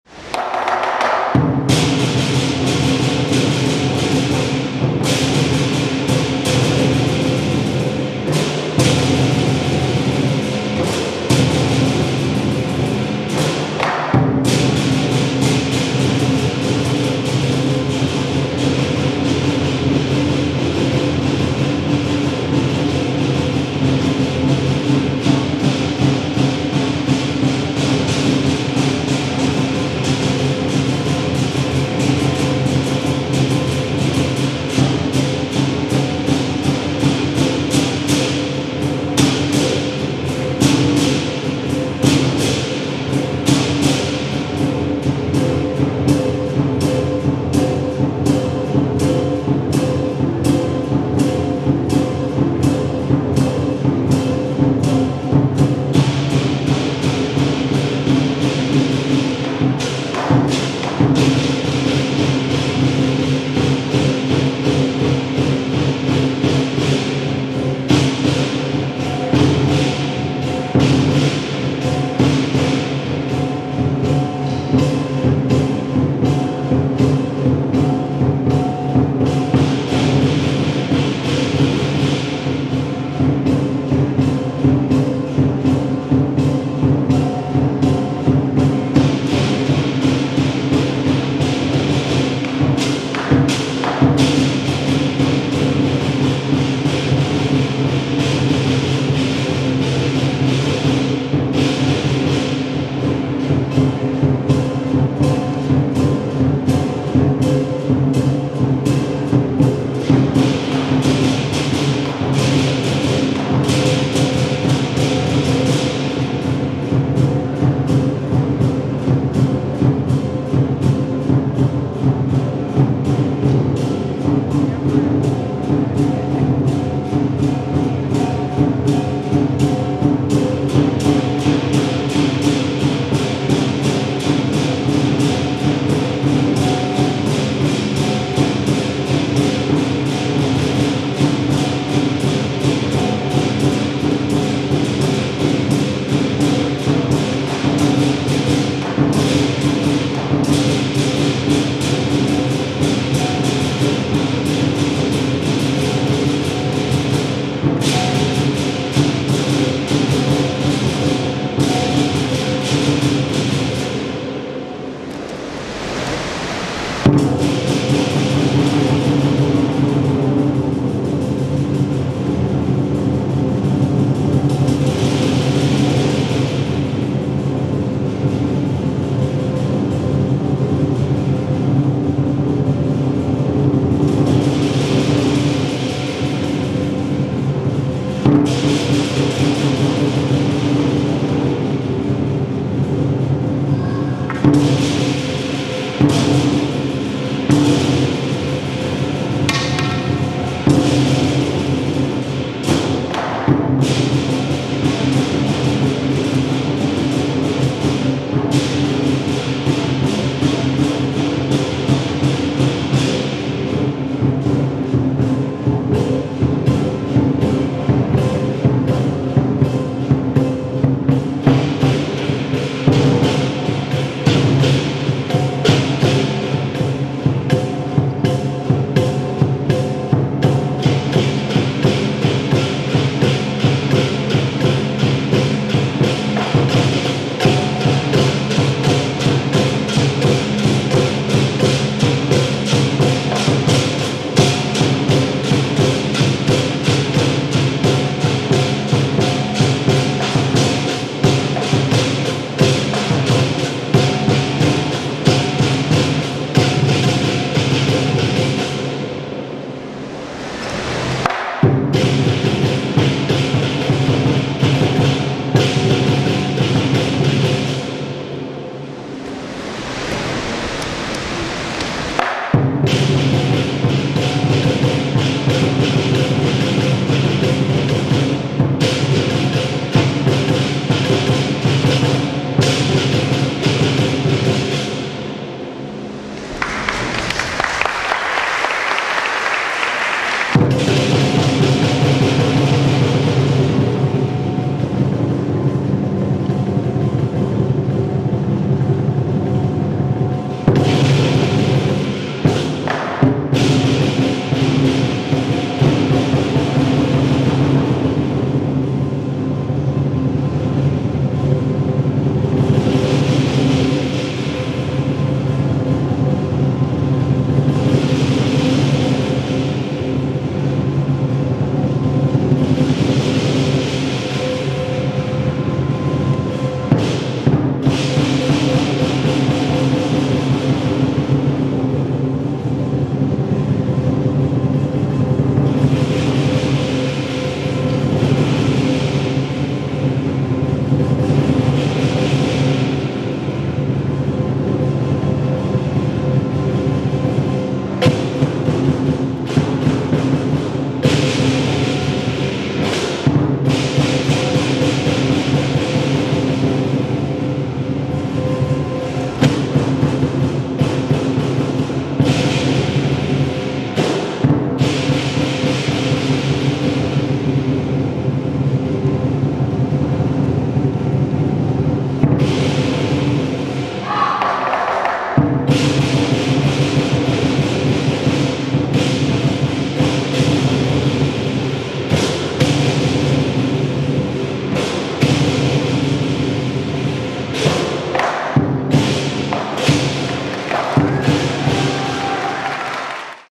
公教中学醒狮团A队
2011年马来西亚雪州校际传统南狮（器材）邀请赛